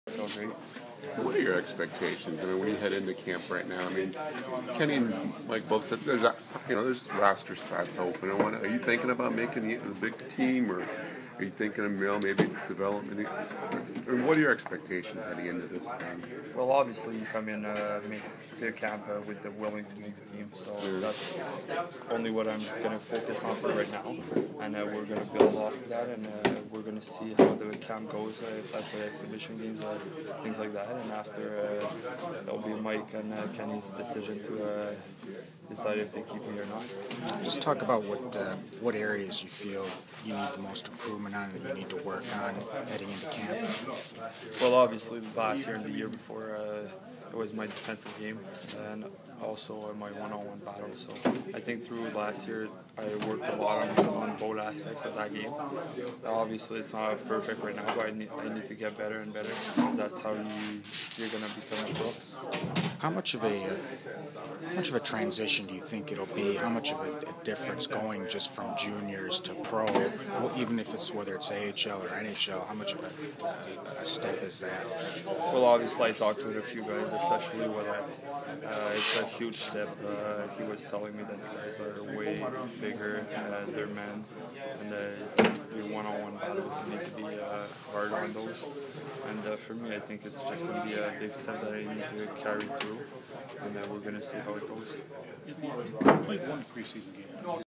Anthony Mantha skates at Red Wings informal practice
Red Wings top prospect Anthony Mantha joined several players at Joe Louis Arena Friday for an informal skate.